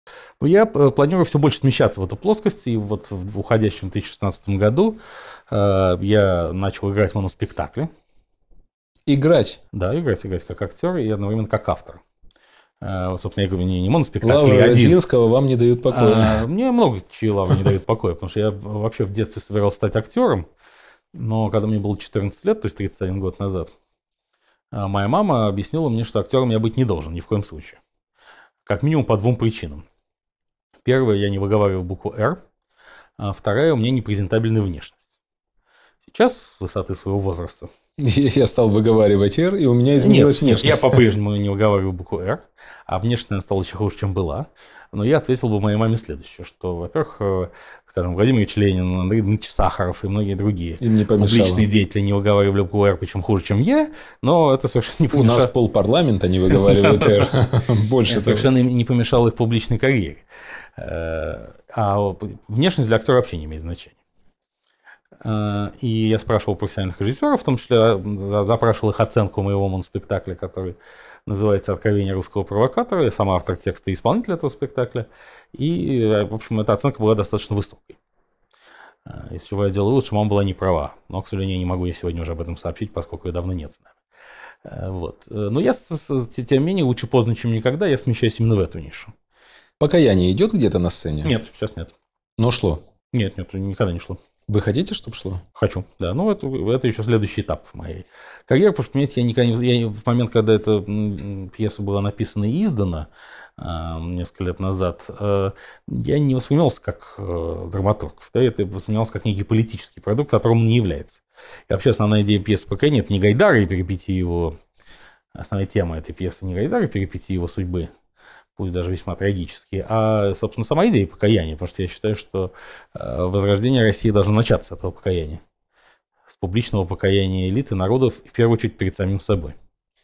Белковский: В детстве мама мне объяснила, что я не должен мечтать о сцене
Фрагмент интервью Дмитрия Гордона с российским политологом Станиславом Белковским. 2016 год